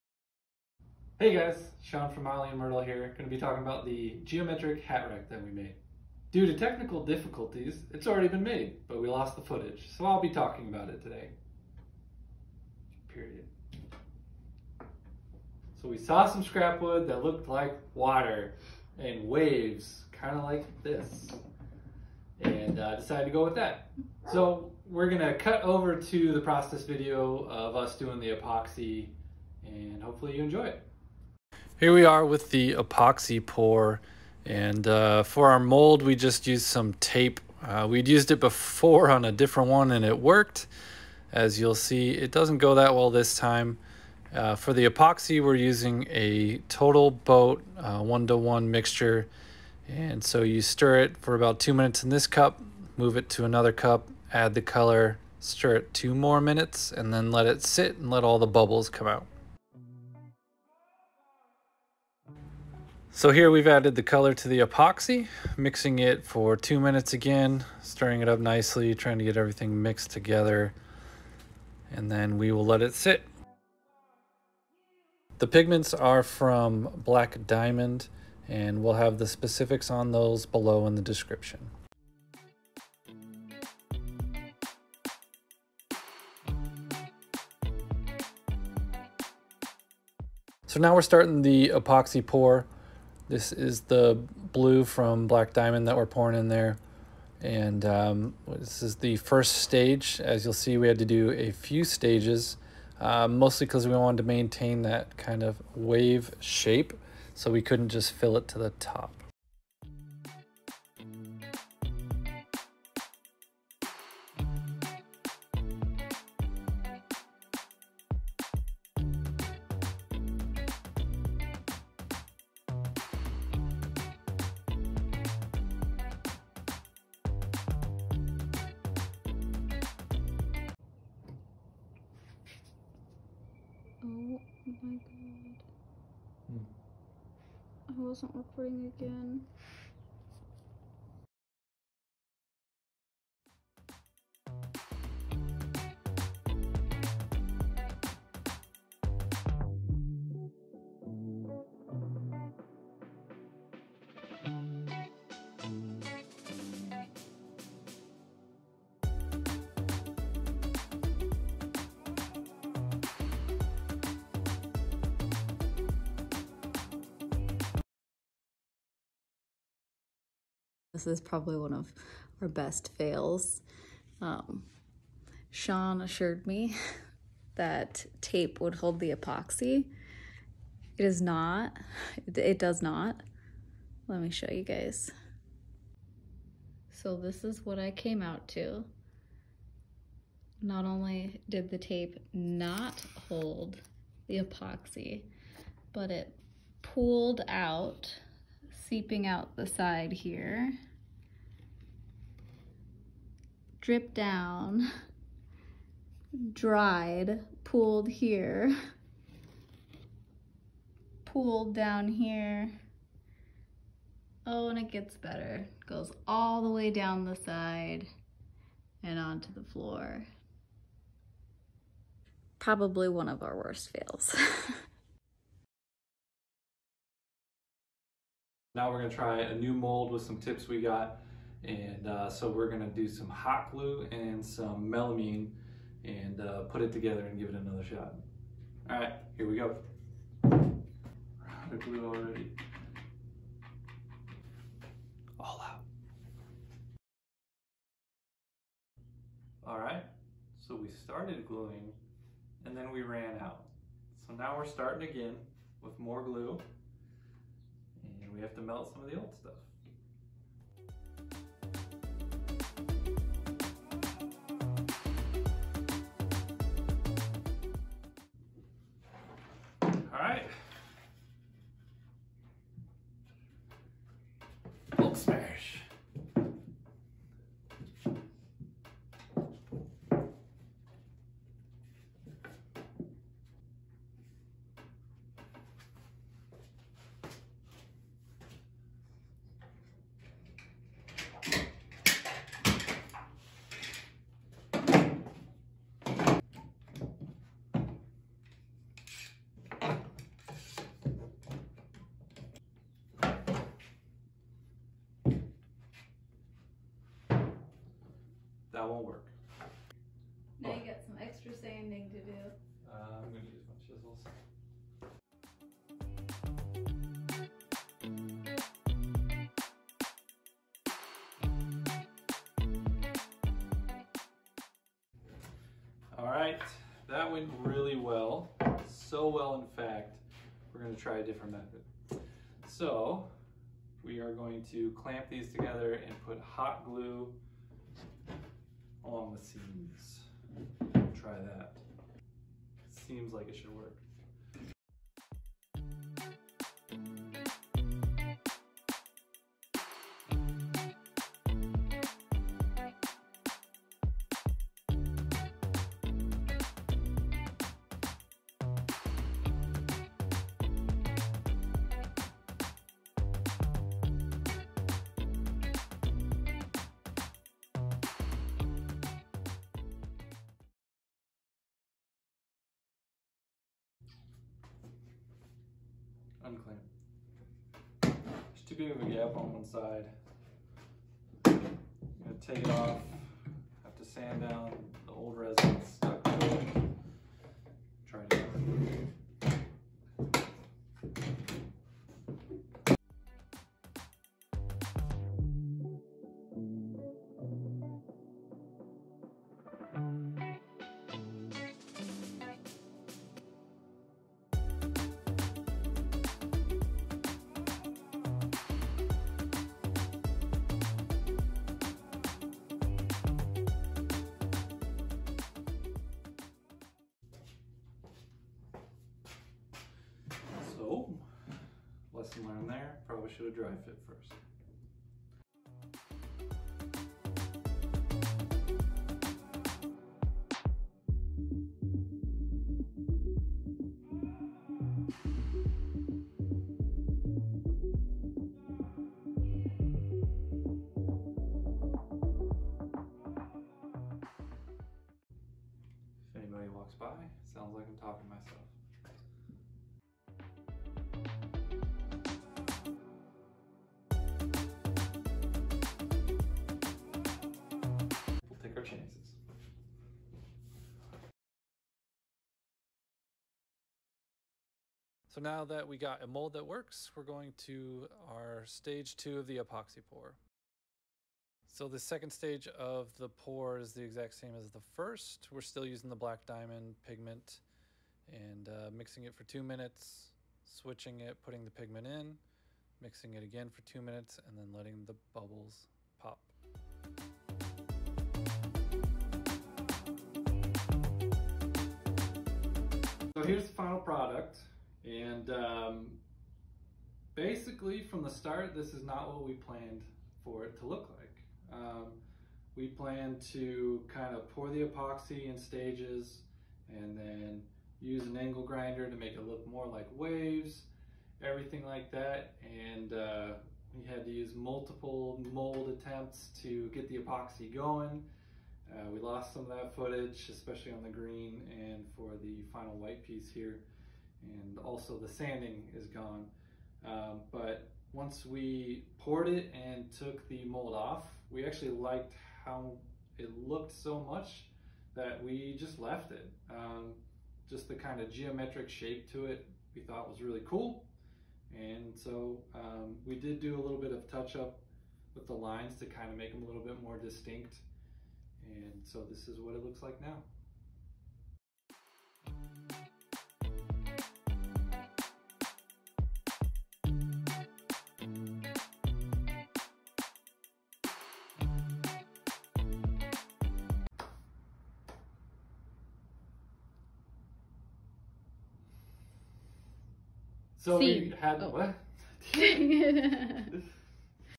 DIY Hat Rack // Making a Geometric Hat Rack // Hat Hanger Build
A little late but for the month of March we were challenged with making something using the theme of water. Using scrap wood and epoxy, we created a geometric wave hat rack.

Materials:

- Scrap wood
- Total Boat - Epoxy Resin Crystal Clear Resin & Hardener (link to product in Amazon Store
- Color Pigment
- Melamine for the mold
- Hot glue

Black Diamond Pigments Used:

- Blue color was called "Iridescent Blue"
- Green color was called "Iridescent Green"
- White color was called "Pure White"

Tool Recommendations